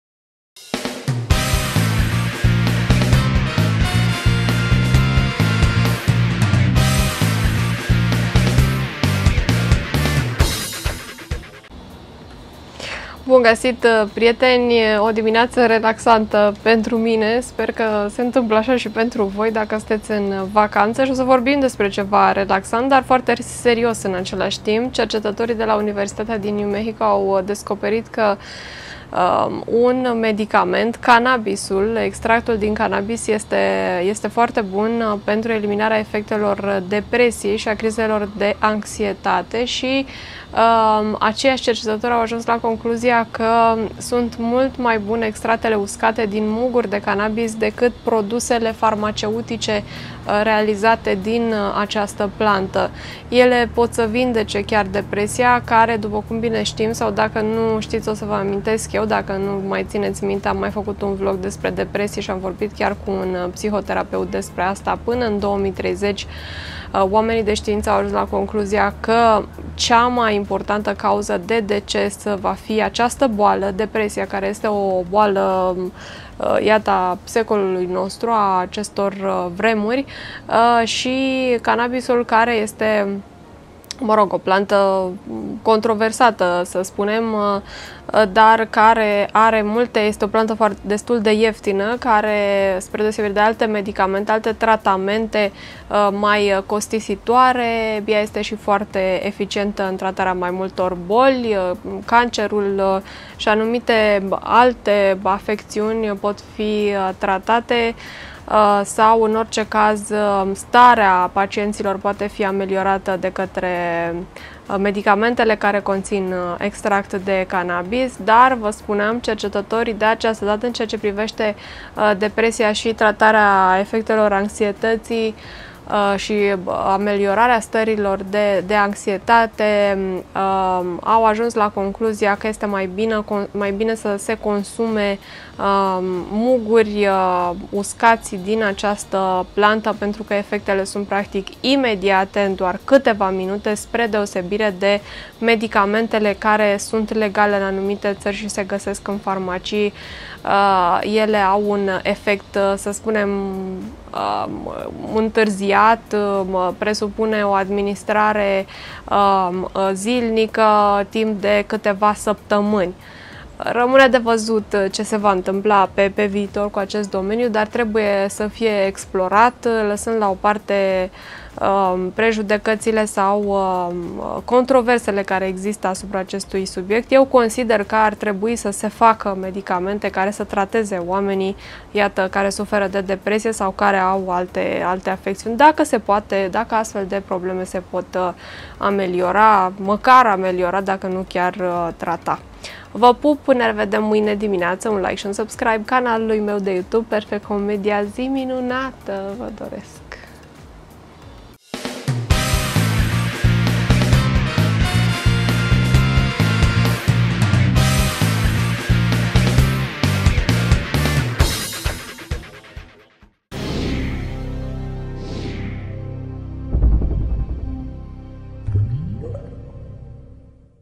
Canabisul vindeca rapid multe boli
De vorba cu Iulia! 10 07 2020